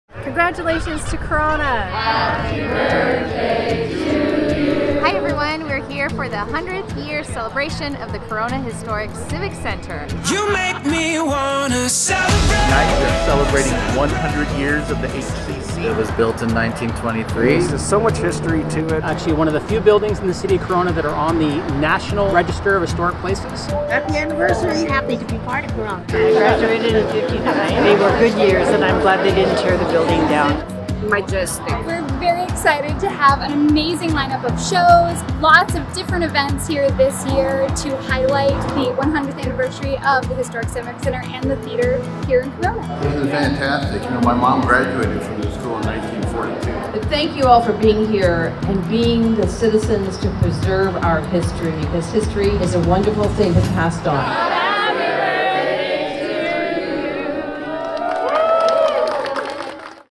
100th Anniversary of our City's Historic Civic Center 🎂✨
🎉 A heartfelt thank you for joining us in commemorating the momentous 100th Anniversary of our City's Historic Civic Center last Thursday, Sept. 28th! 🎂✨
This event was a celebration of the HCC's rich history and a chance to unite the incredible individuals who call this place home. 🏛️❤️
Explore the exciting lineup of events taking place at the HCC